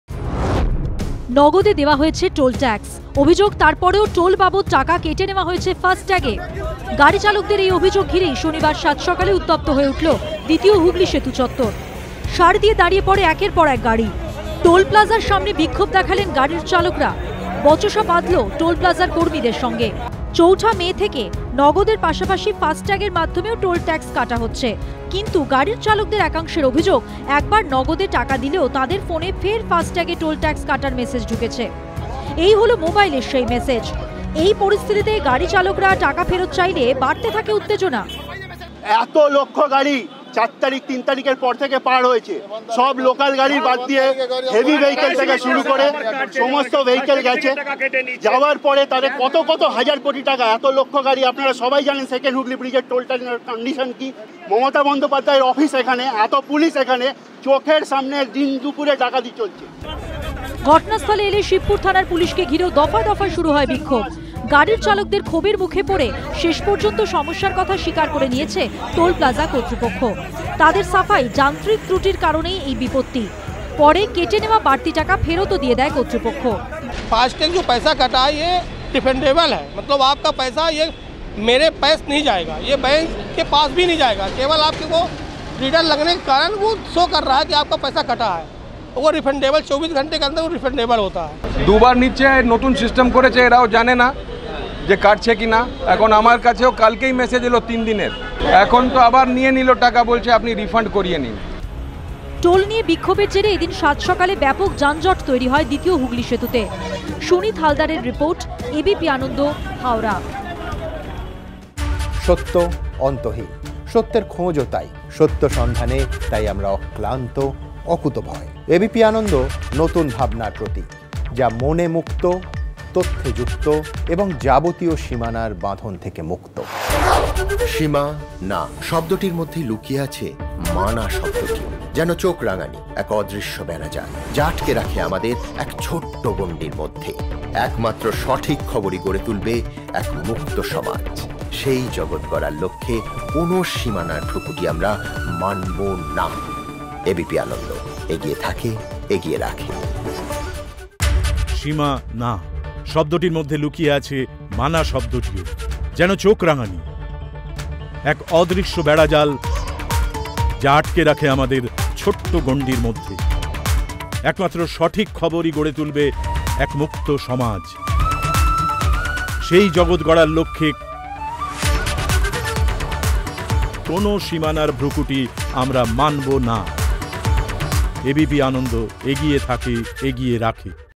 Hooghly Bridge:দ্বিতীয় হুগলি সেতুতে টোল ট্যাক্স নেওয়াকে কেন্দ্র করে উত্তেজনা ।Bangla News
নগদের পাশাপাশি ফাস্ট্যাগের মাধ্যমেও কাটা হচ্ছে টোল ট্যাক্স? দ্বিতীয় হুগলি সেতুতে উত্তেজনা